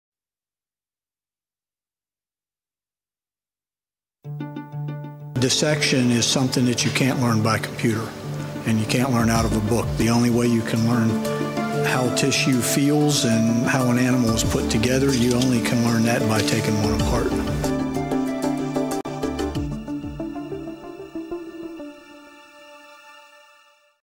Pre-Medical Laboratory
Students and Faculty at Weill Cornell Medical College in Qatar share their insights and talk about their experience at the college.